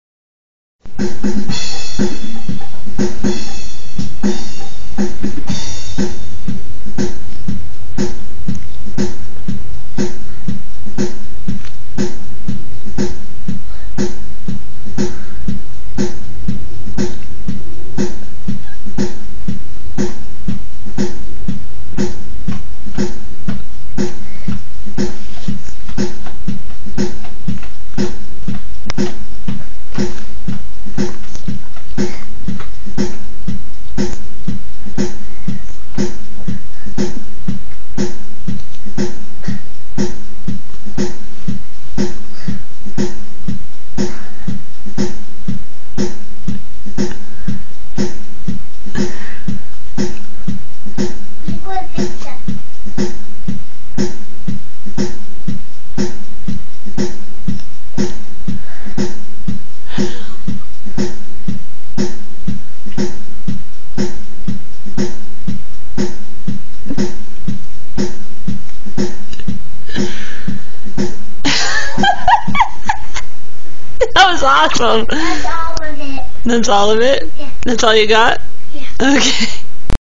More Landon Dancing
I can watch Landon dancing all day long! The kid makes me laugh.